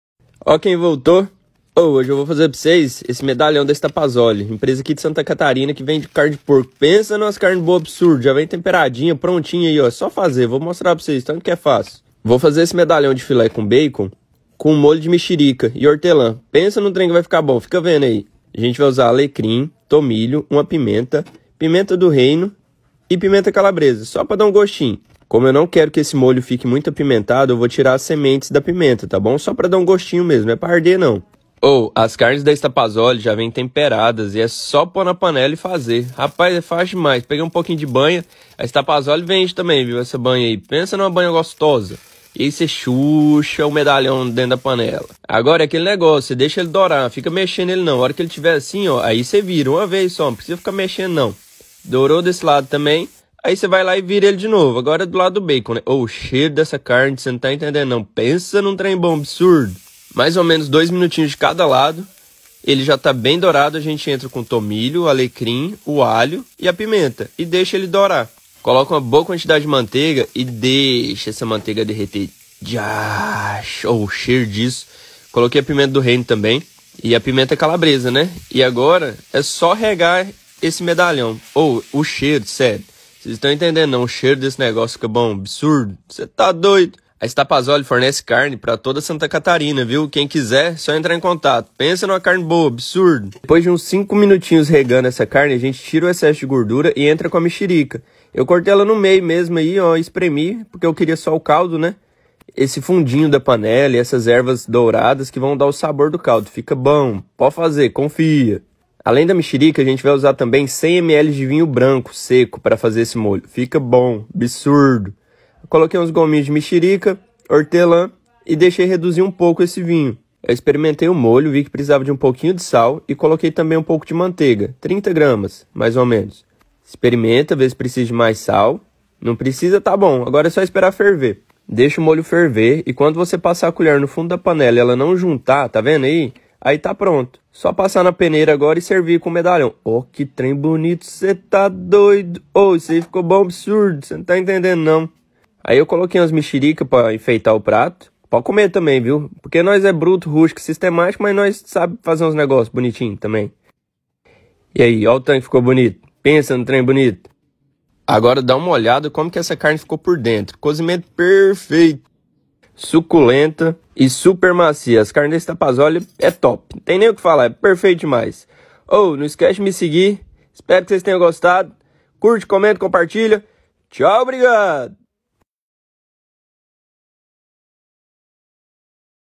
Medalhão de filé de porco com molho de mexerica
Ingredientes:

1 pacote de medalhão de filé de porco
3 mexericas
100ml de vinho branco seco
1 galho de alecrim
2 galhos de tomilho
2 dentes de alho
1 pitada de pimenta do reino
1 pitada de pimenta calabresa
1 pitada de sal
4 folhas de Hortelã
1 pimenta dedo de moça
50g de manteiga de leite
20g de banha de porco

Depois disso é só alegria!! Tchau brigado!